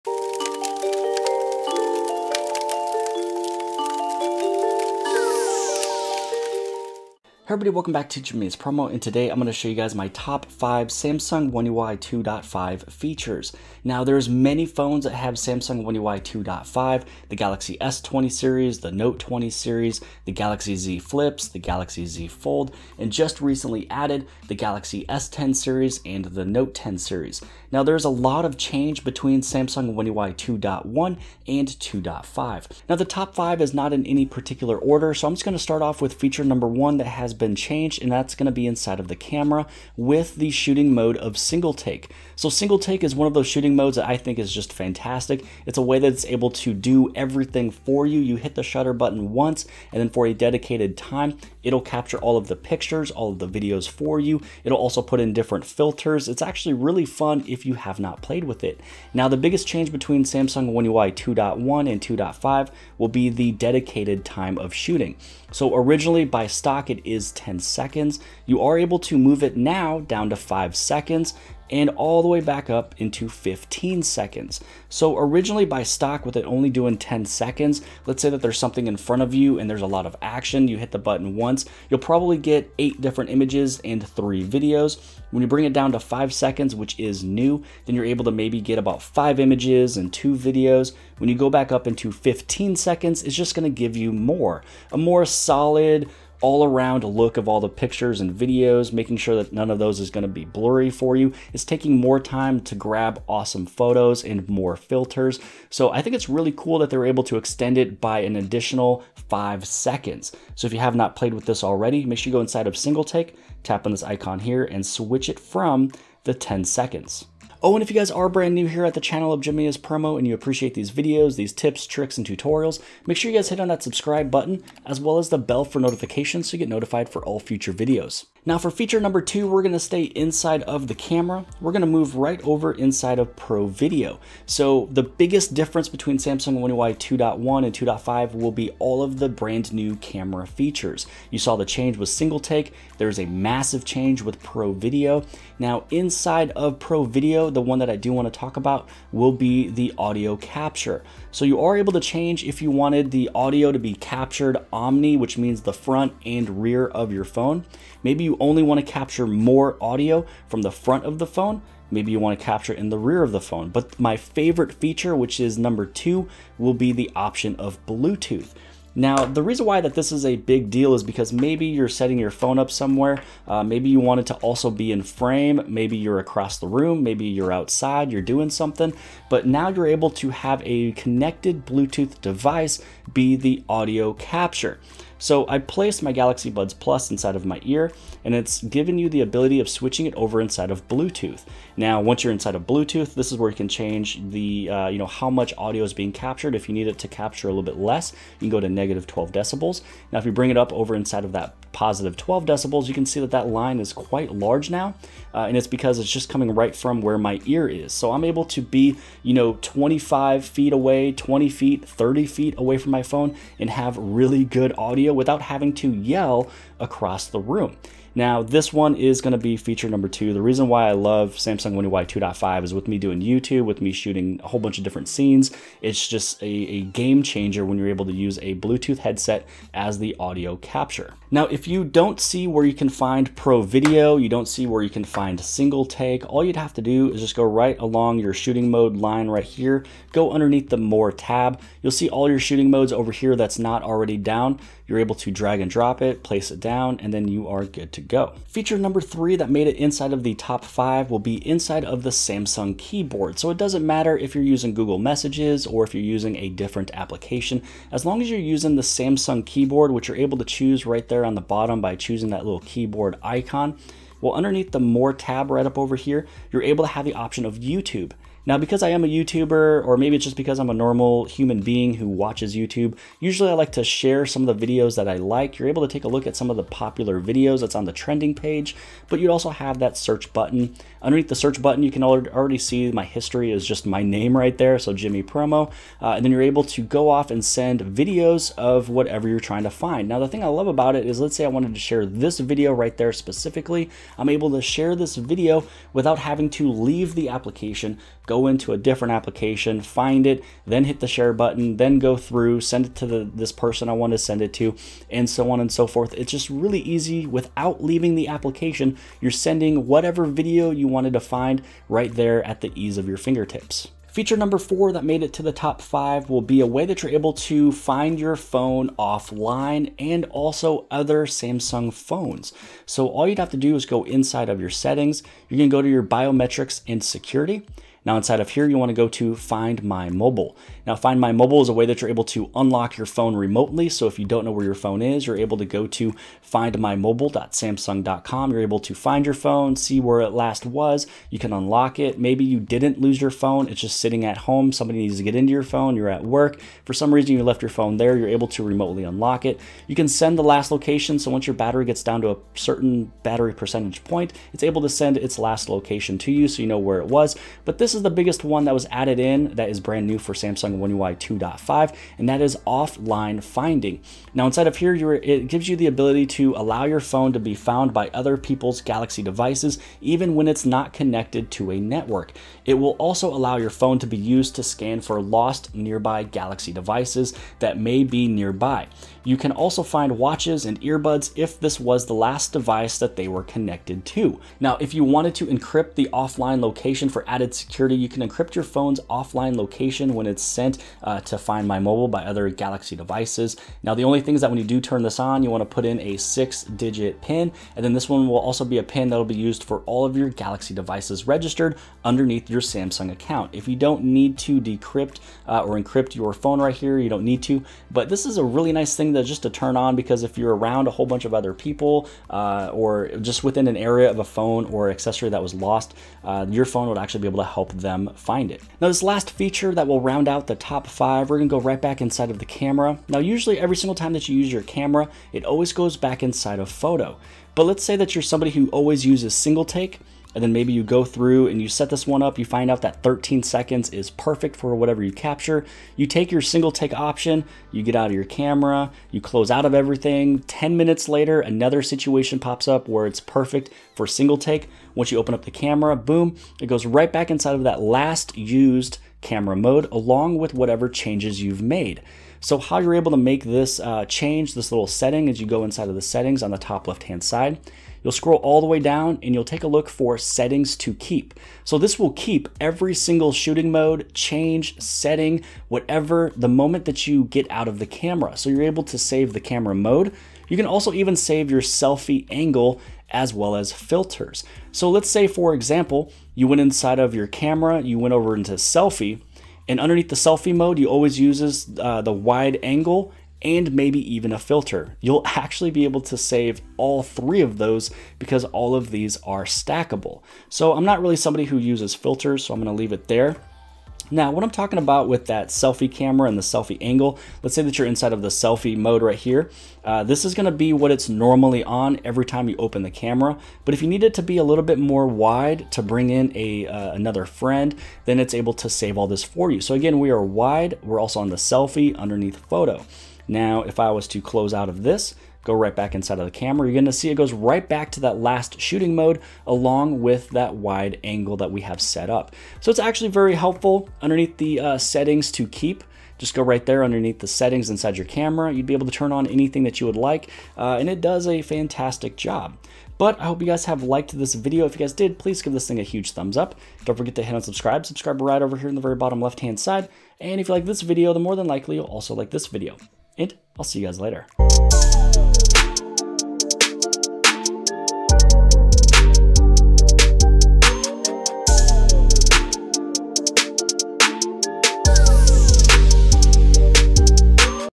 Samsung One UI 2.5 - TOP 5 FEATURES
Welcome to the home of the best How-to guides for your Samsung Galaxy needs. In today's video, we go over the top 5 features apart of Samsung One UI 2.5. This is a short list of my favorite features, not in any particular order.

Note 20 Ultra Case In Video (Choose Your Color)